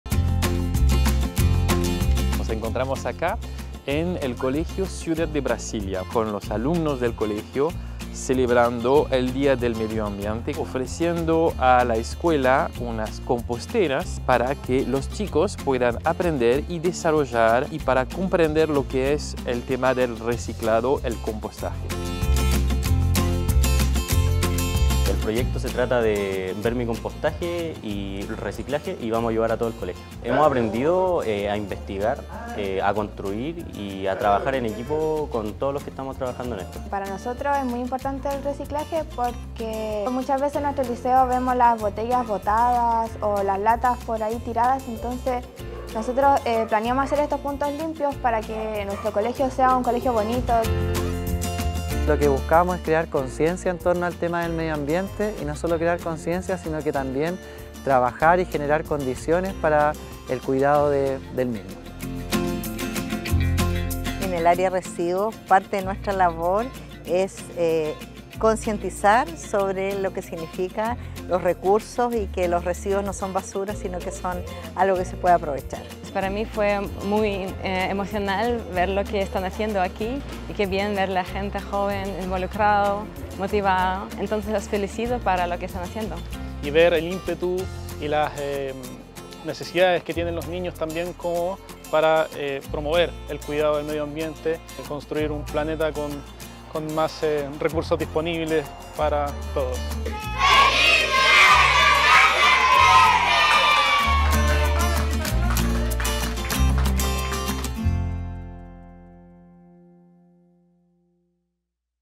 Nuevo Pudahuel y Liceo Ciudad de Brasilia celebran el Día del Medio Ambiente con foco en reciclaje
Con el fin de seguir estrechando lazos con las comunidades vecinas del Aeropuerto de Santiago, Nuevo Pudahuel realizó una nueva jornada de colaboración junto al Liceo Ciudad de Brasilia, ubicado a sólo 6 kilómetros del terminal aéreo, en la localidad de Noviciado. Liderado por el equipo de Comunicaciones & Asuntos Corporativos y la Gerencia de Sustentabilidad de Nuevo Pudahuel, la actividad se enmarcó en el Día del Medio Ambiente.
En la instancia participaron Veolia Chile, quien ejecuta el plan de recuperación de residuos en el Aeropuerto de Santiago, compartiendo un taller para 35 alumnos de cuarto básico para abordar la importancia del reciclaje en el ámbito escolar y doméstico en el cuidado de nuestro planeta.
Asimismo, se contó con la participación de representantes del Programa de Naciones Unidas para el Medio Ambiente tanto de Chile como también de Europa (Austria), quienes compartieron con los/as estudiantes y aprendieron sobre sus planes de construcción de un invernadero, donde será clave contar con abonos naturales provenientes de residuos orgánicos.
Precisamente en este último aspecto, Nuevo Pudahuel y Veolia donaron cuatro composteras, lo que ayudará a mejorar la fertilidad de la tierra, ya que el compost es un gran alimento para las plantas.